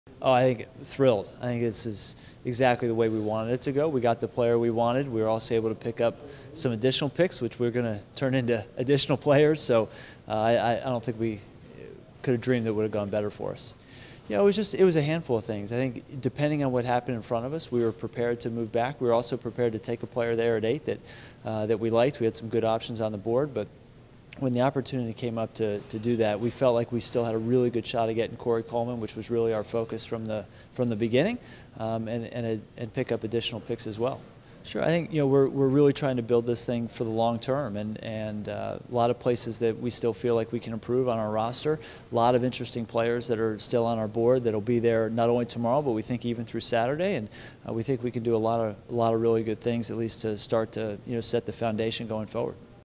DePodesta: "Exactly the way we wanted it to go"
Chief Strategy Officer Paul DePodesta talks about the 1st round of the NFL Draft and looks ahead to the rest of the draft.